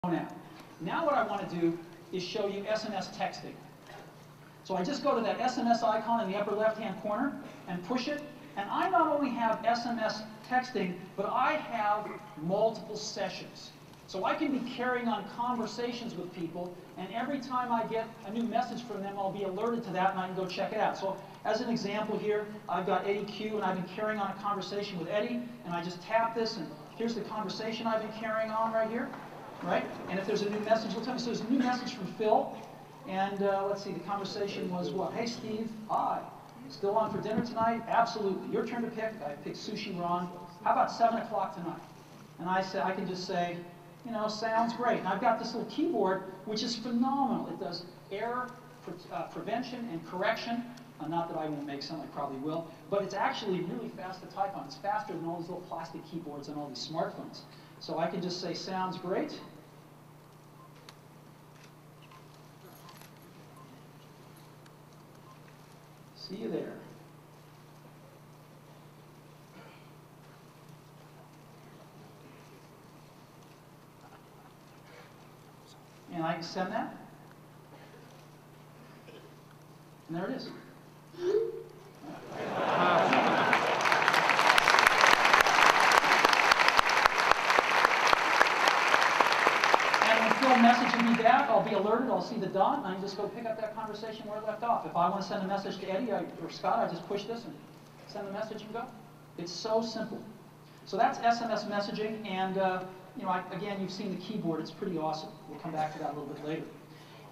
MYiPhone: iPhone Demo of SMS Text - virtual QWERTY Keyboard
Great video showing how SMS and the virtual keyboard works on the iPhone